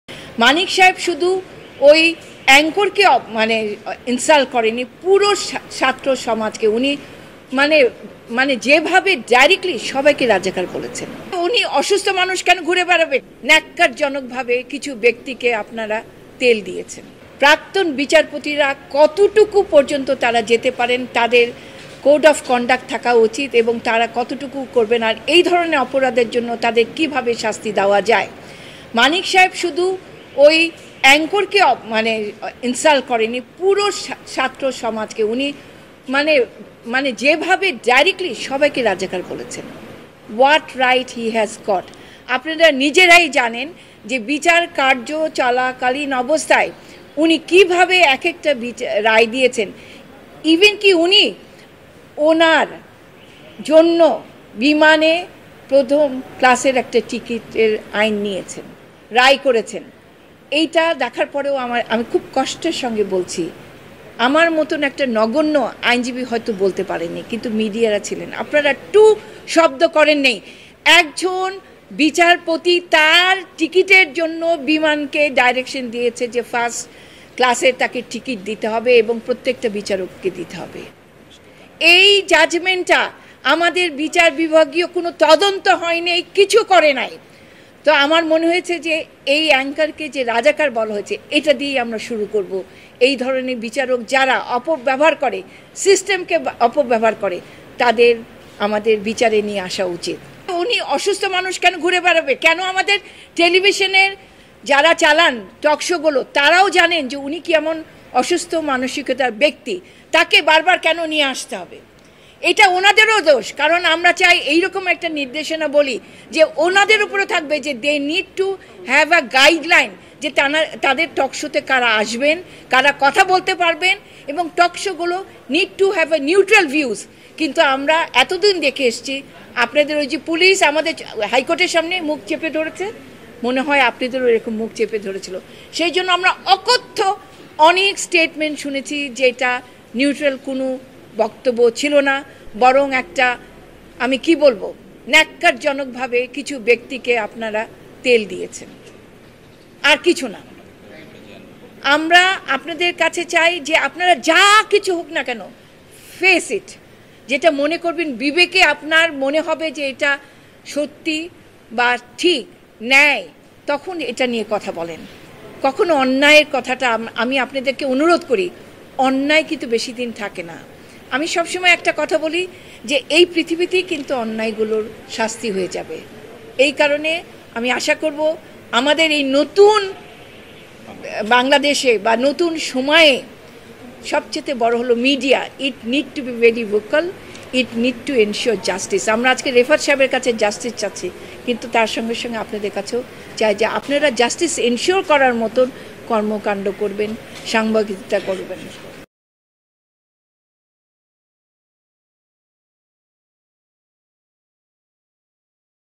বিচারপতি মানিকের বিরুদ্ধে লিগ্যাল নোটিশ জারি হচ্ছে! | Justice Manik | High Court | Maasranga News
বিচারপতি মানিকের বিরুদ্ধে লিগ্যাল নোটিশ জারি হচ্ছে! | Justice Manik | High Court

2 Bir Uttam Ziaur Rahman Road (New Airport Road)